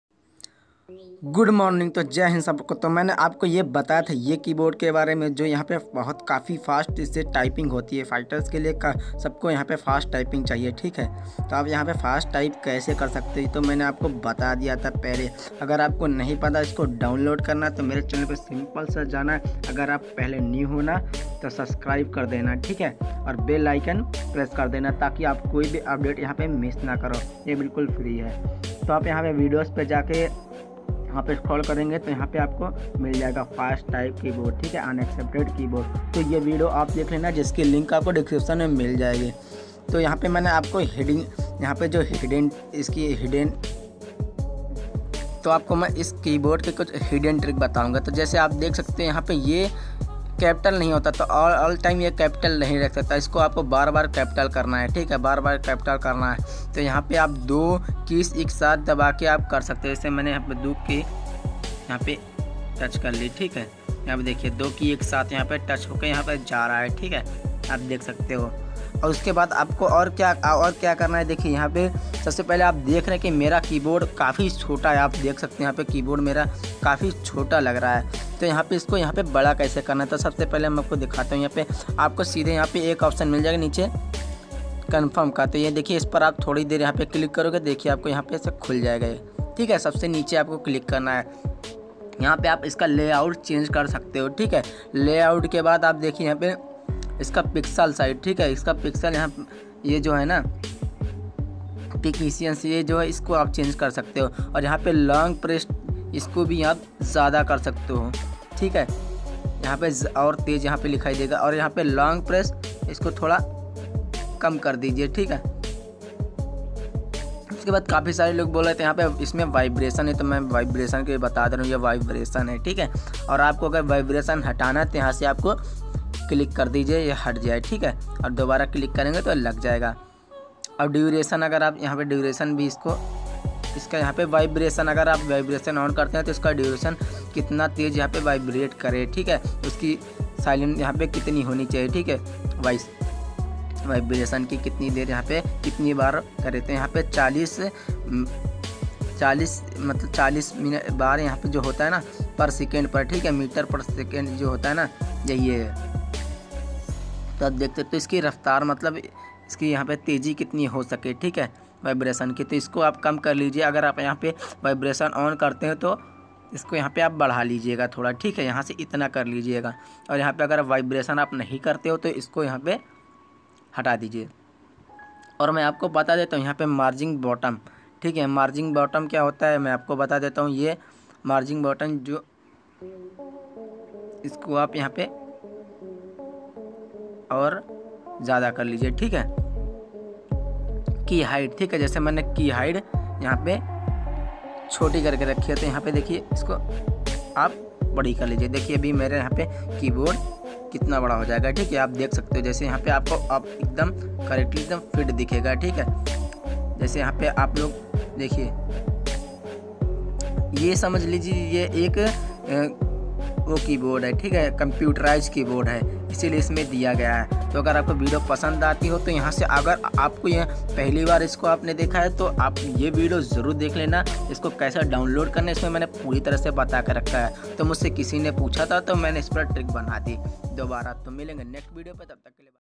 Top hiden trick On Unexpected keyboard | How to use and Download unexpected keybord indaintricker
MD SHAHNWAZ FT DARINDAA ZINN

FB TRICK PAGE KA LINK

FB TRICKGROUP KA LINK

MY INSTAGRAM ID KA LINK